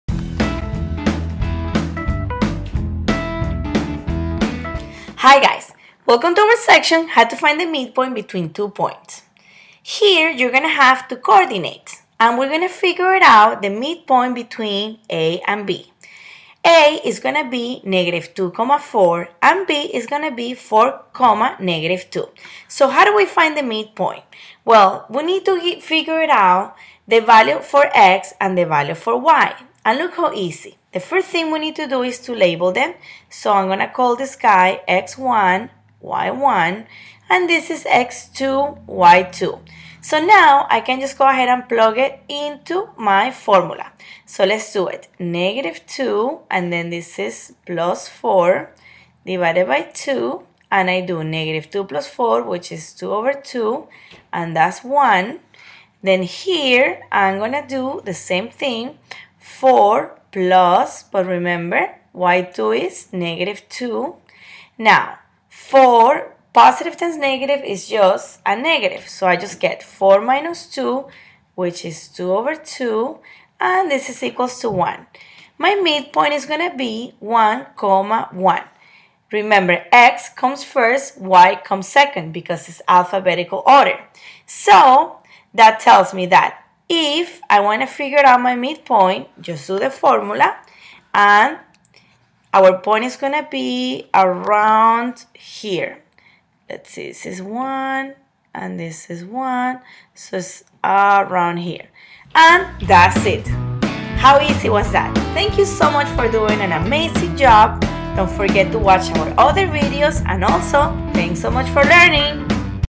How to find the midpoint between two points
Hi Guys,

This video will show you how to find the midpoint between two points